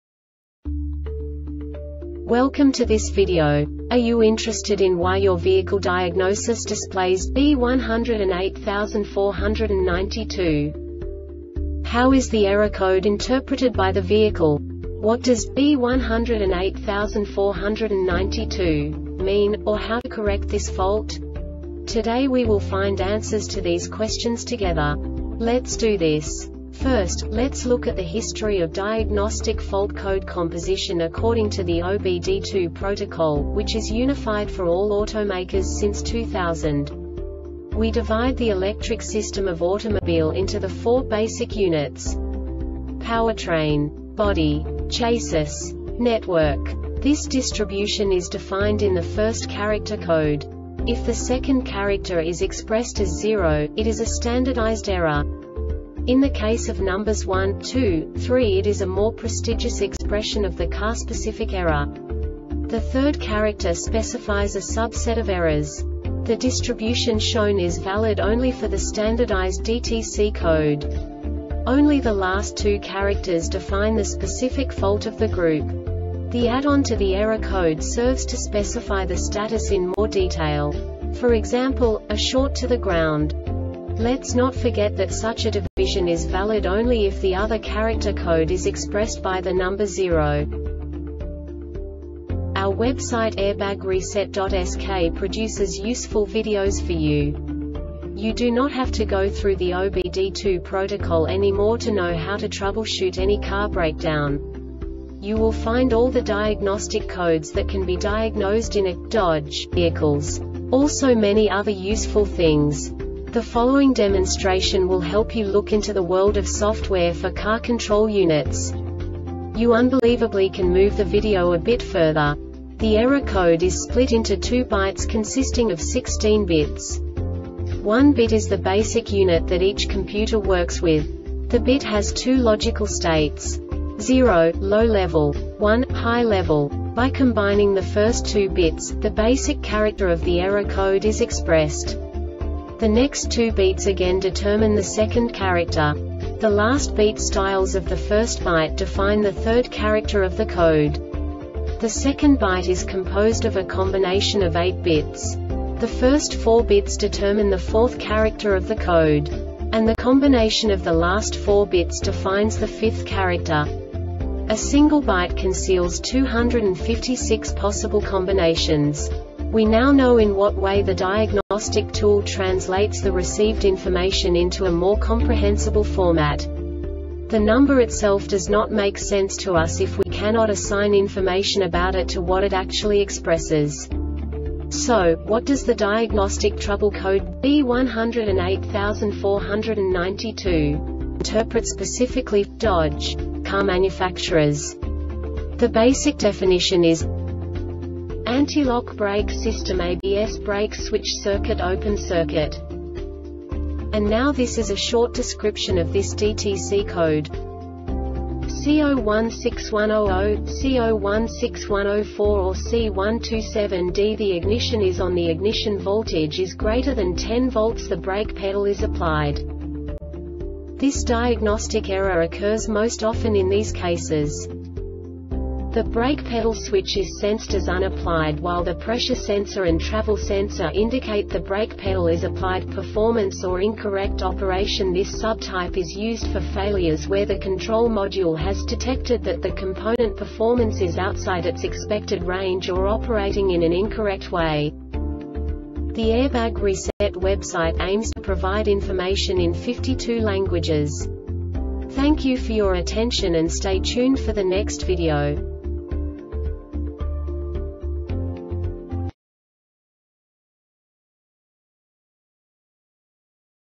DTC Dodge B1084-92 Short Explanation
Contents:
0:21 Basic DTC analysis according to OBD2 protocol standard.
1:48 Insight into programming
2:58 A diagnostically understandable description of the Diagnostic Trouble Code
3:05 Basic error definition

Make
Dodge
Code
B1084 92
Definition
Antilock Brake System ABS Brake Switch Circuit Open Circuit
Description
C0161 00, C0161 04 or C127D The ignition is ON  The ignition voltage is greater than 10 volts  The brake pedal is applied
Cause
The brake pedal switch is sensed as unapplied while the pressure sensor and travel sensor indicate the brake pedal is applied
Failure Type